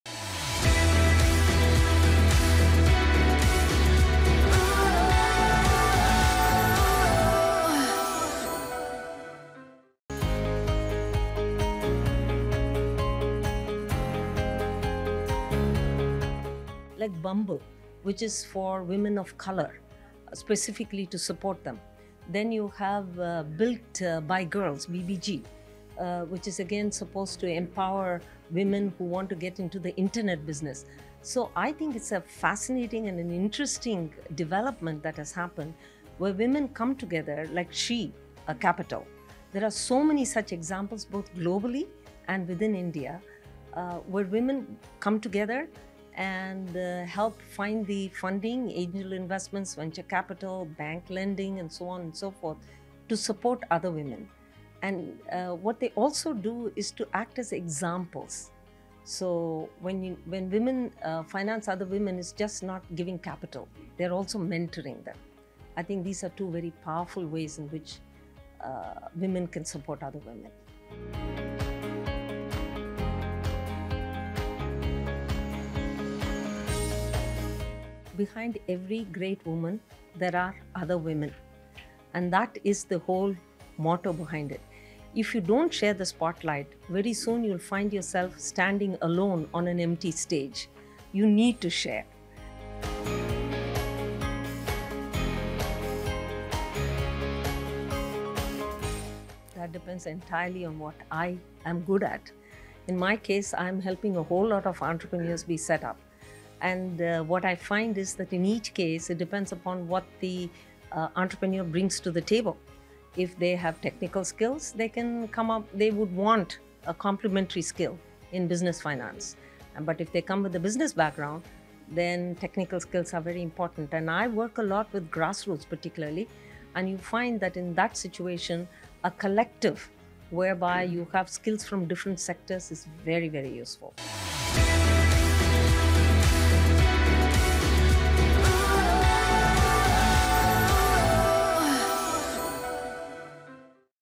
Storyboard18’s ShareTheSpotlight | Lakshmi Venkatraman Venkatesan Talks About Women Uplifting Women
Lakshmi Venkatraman Venkatesan, Founding & Managing Trustee of Bharatiya Yuva Shakti Trust, highlights the power of women coming together to support, finance, mentor, and empower other women at Storyboard18’s ShareTheSpotlight. Take a look at her inspiring words!